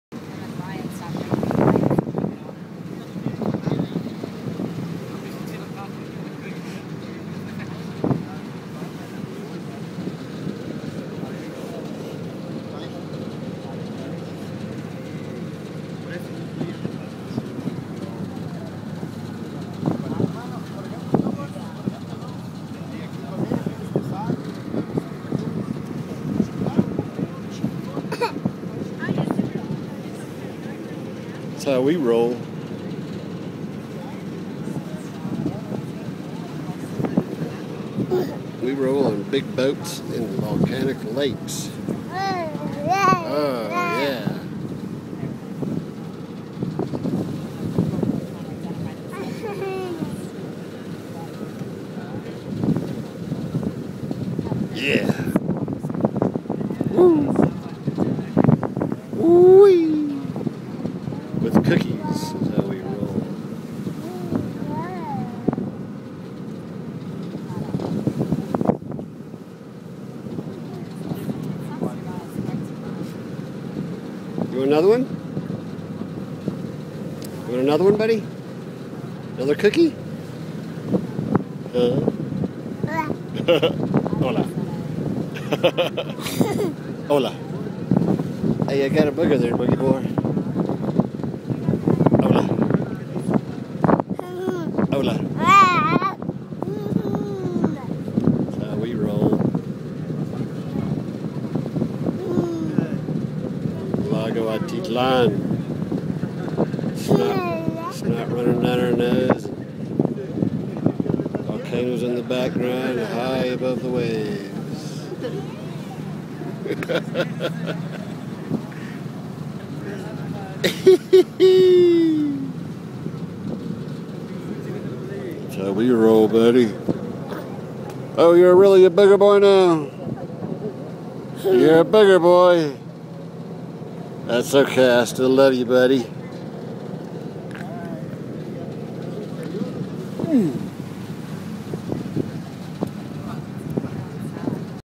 Booger Boat Boy
On top of ferry boat on Lake Atitlan w Orion at 16 months